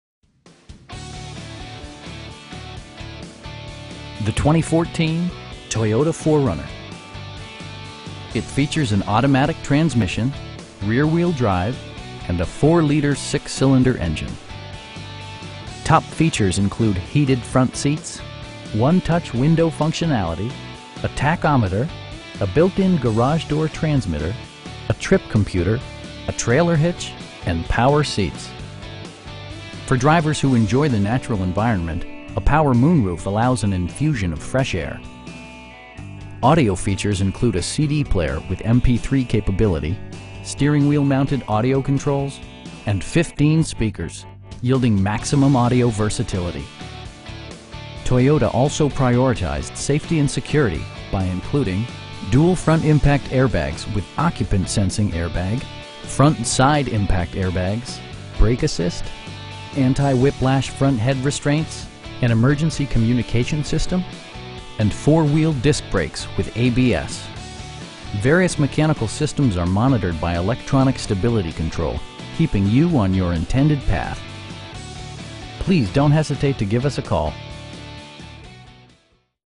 2014 Toyota 4Runner Limited in North Little Rock, AR 72117
The 2014 Toyota 4Runner. It features an automatic transmission, rear-wheel drive, and a 4 liter 6 cylinder engine. Top features include heated front seats, 1-touch window functionality, a tachometer, a built-in garage door transmitter, a trip computer, cruise control, a trailer hitch, and power seats. Features such as automatic climate control and leather upholstery prove that economical transportation does not need to be sparsely equipped. For drivers who enjoy the natural environment, a power moon roof allows an infusion of fresh air. Audio features include a CD player with MP3 capability, steering wheel mounted audio controls, and 15 speakers, providing excellent sound throughout the cabin. Toyota also prioritized safety and security by including: dual front impact airbags with occupant sensing airbag, front SIDE impact airbags, traction control, brake assist, anti-whiplash front head restraints, a security system, an emergency communication system, and 4 wheel disc brakes with ABS. Various mechanical systems are monitored by electronic stability control, keeping you on your intended path. We pride ourselves in the quality that we offer on all of our vehicles. Please don't hesitate to give us a call.